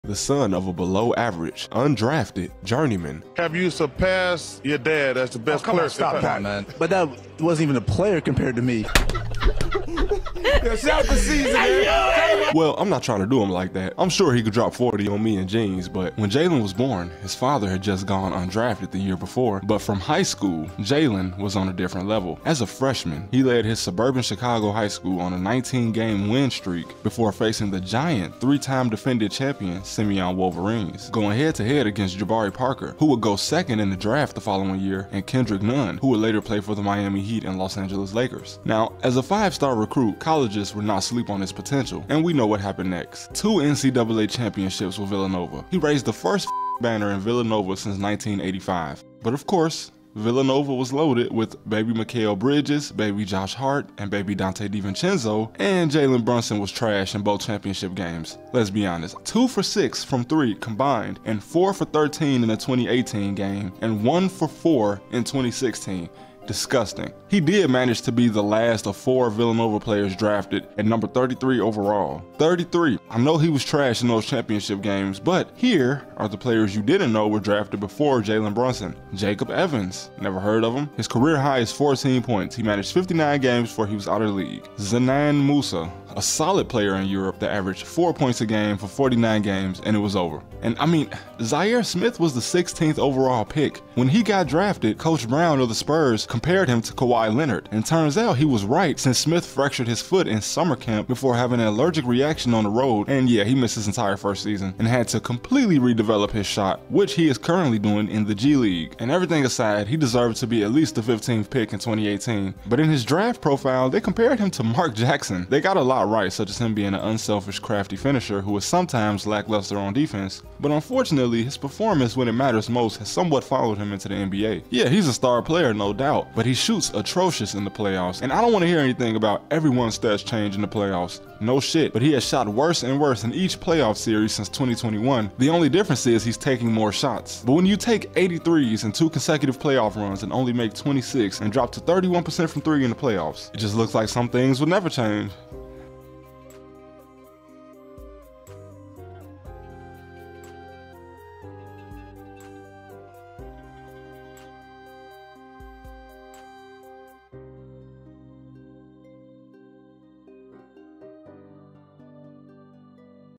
When Your Dad Was Trash But You're an NBA Superstar
In NBA basketball today, we are seeing more son's of NBA players enter the league but in sports, nothing is guaranteed. No matter how good Bronny is, it would be unfair to say he'd ever be like Lebron, but we're talking about New York Knicks star, Jalen Brunson.